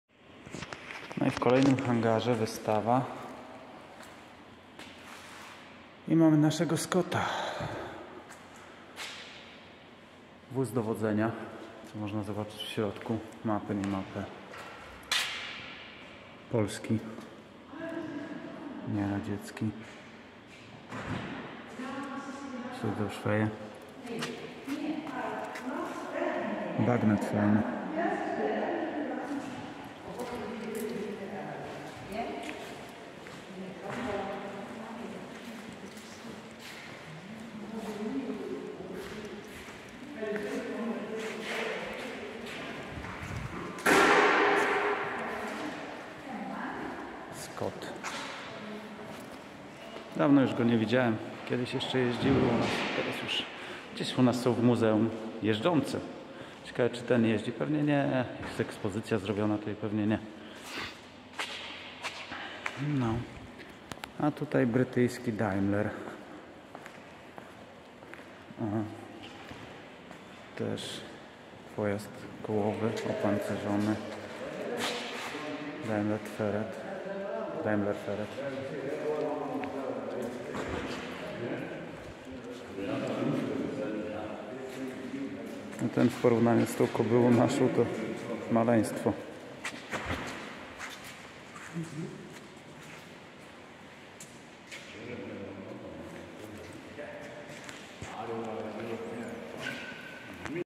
Cold War Museum in Langeland - a large dose of history Part XVIII
Wizyta w muzeum zimnej wojny na Langeland - duza dawka historii i swiadomosci odnosnie polskiej histori znalezione w muzeum w Danii Czesc XVIII